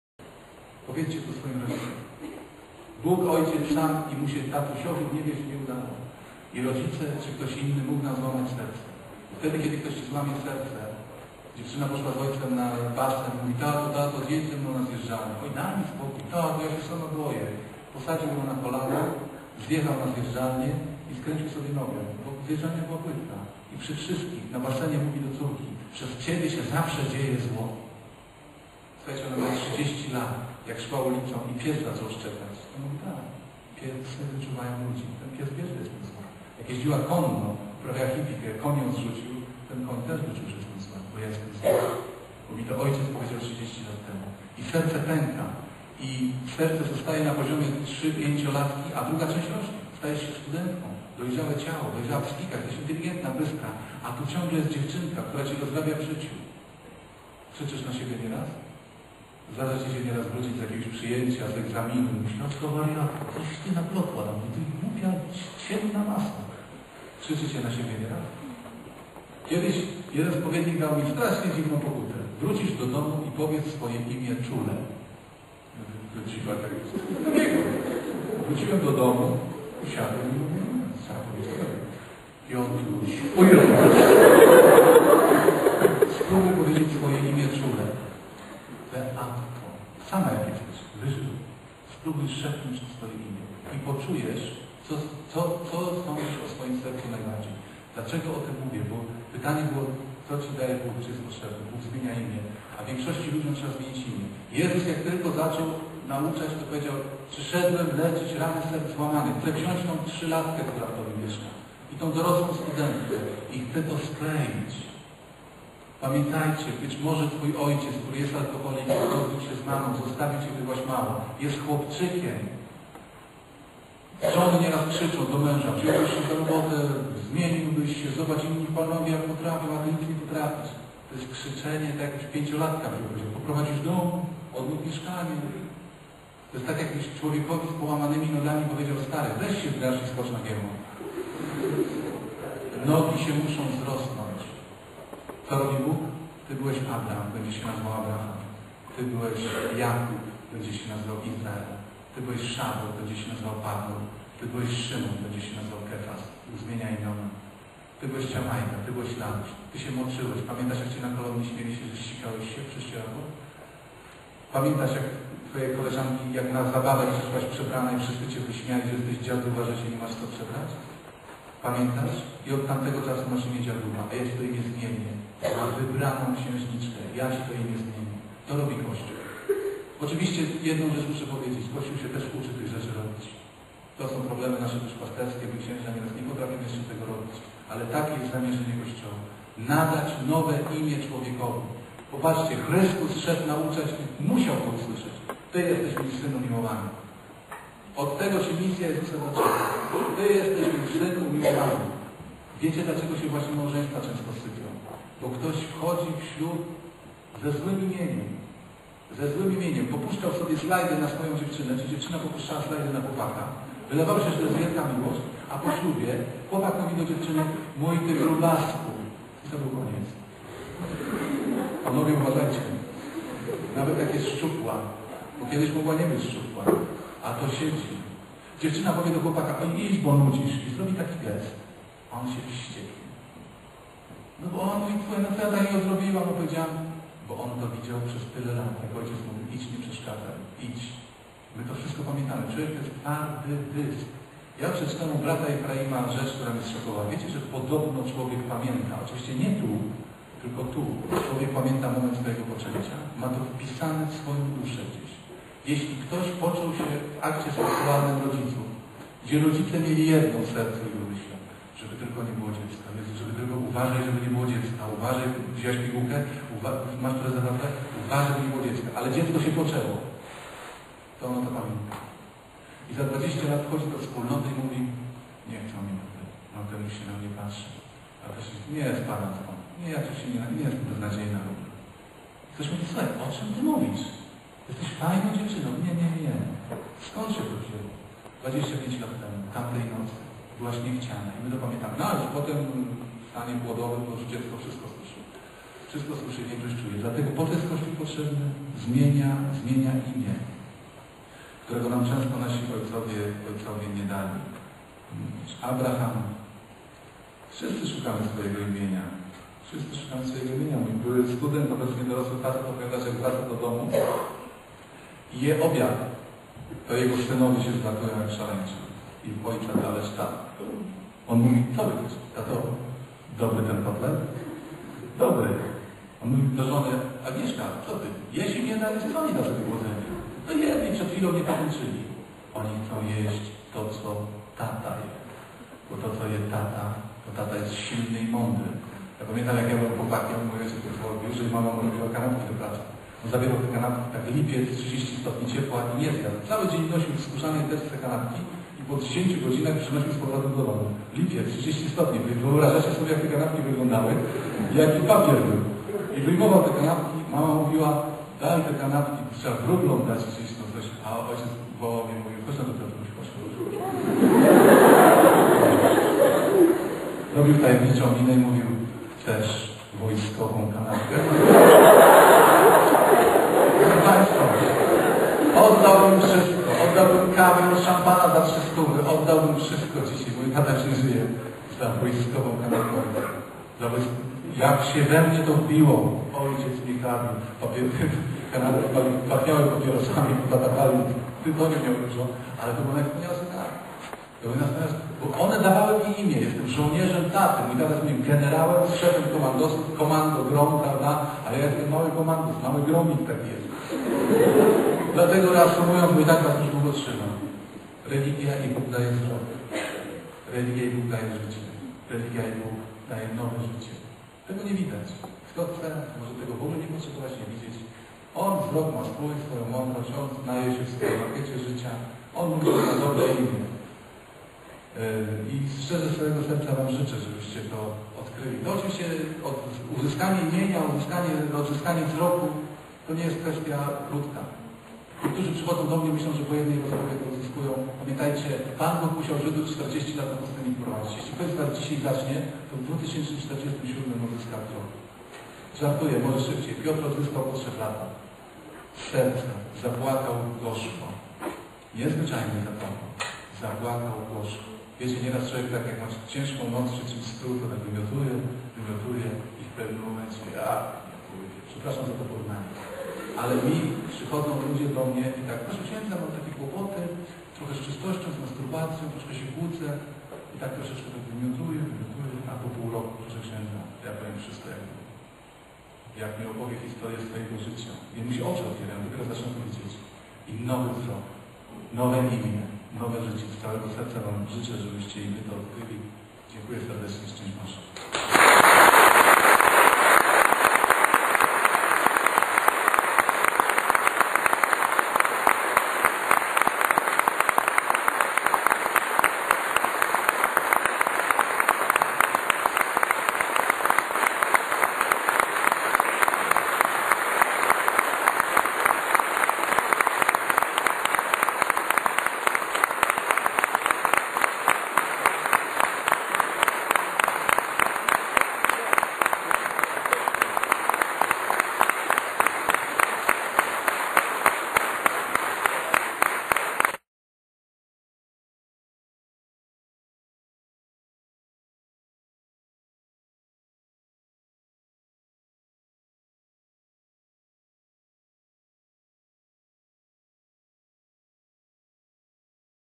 Ks. Pawlukiewicz na AWF w Gdańsku- część 7.(ostatnia)
Wykład/konferencja